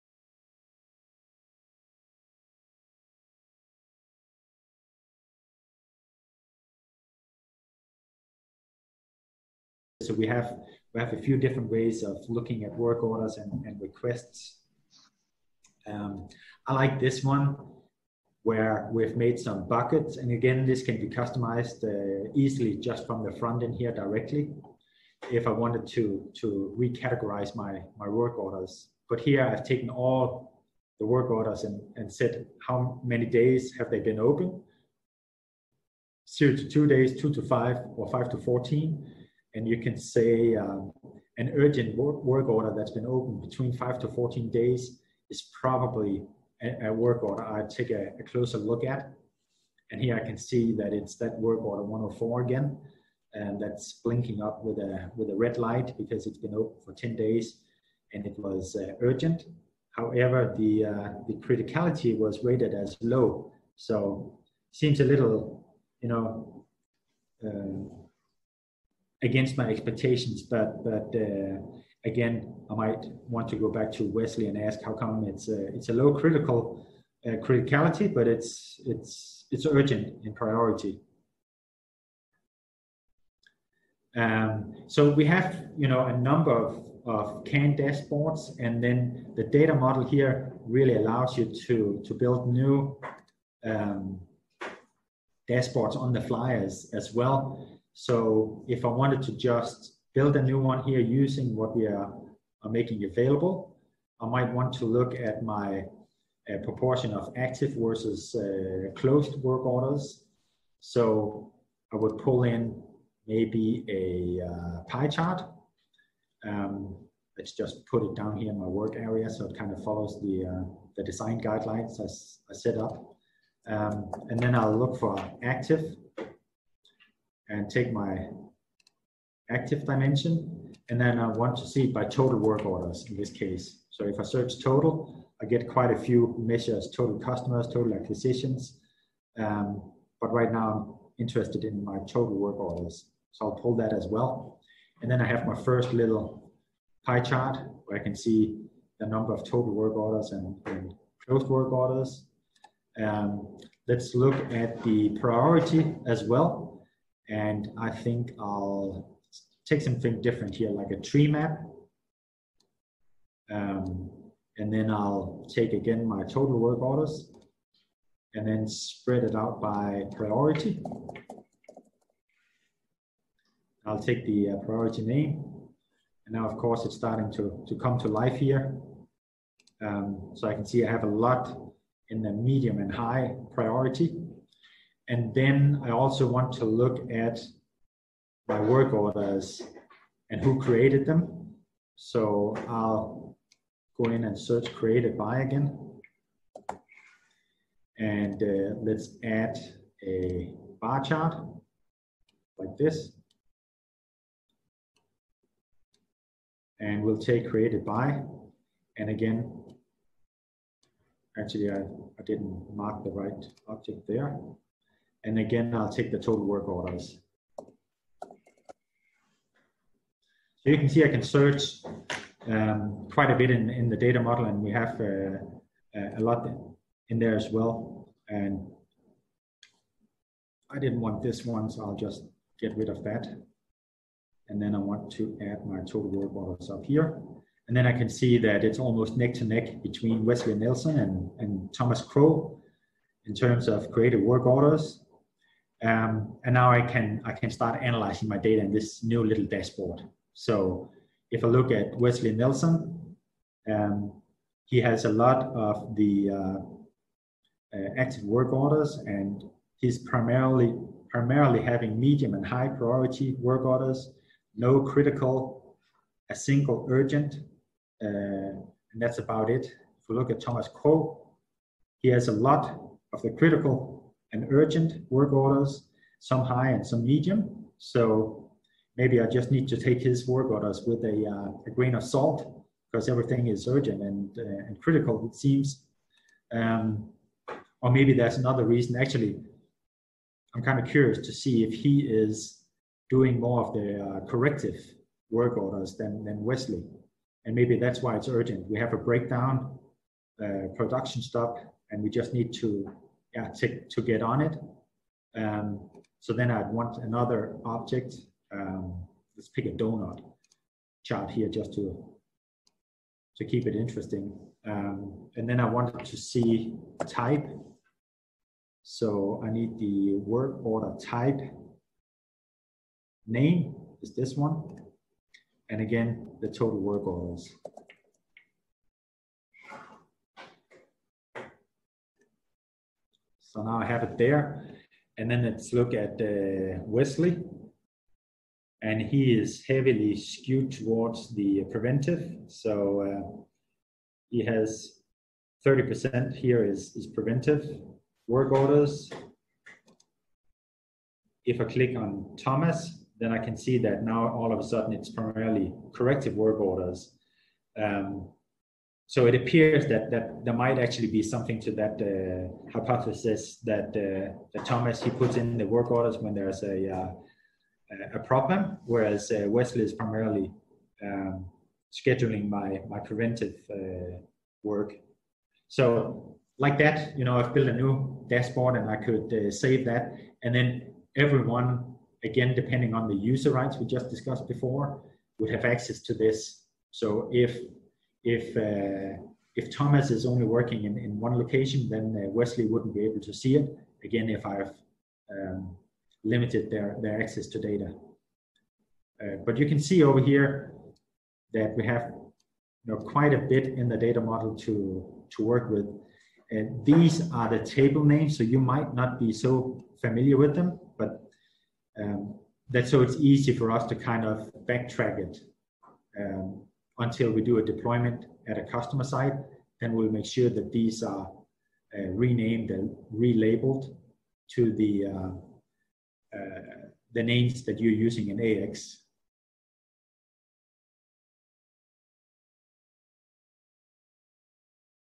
EAM Analytics & Power BI: Work Orders
EAM Analytics can help you answer many daily questions such as:

• How many Work Orders are we opening and how many do we close out
over the day, week or month?

• What is the ratio of preventive maintenance compared to corrective
maintenance?

Dynaway
Enterprise Asset Management solutions
Alfred Nobels Vej 27
Aalborg 9220